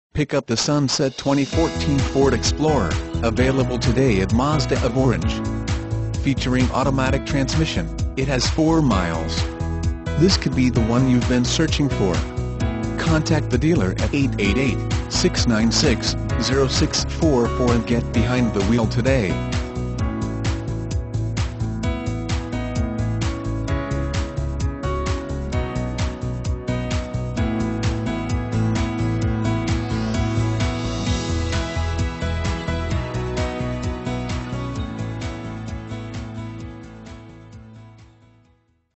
2014 Ford Explorer Orange CA 00440977
2014 Ford Explorer Limited

Pick up this Sunset 2014 Ford Explorer, available today at Mazda of Orange. Featuring Automatic transmission, it has 4 miles. This could be the one you've been searching for.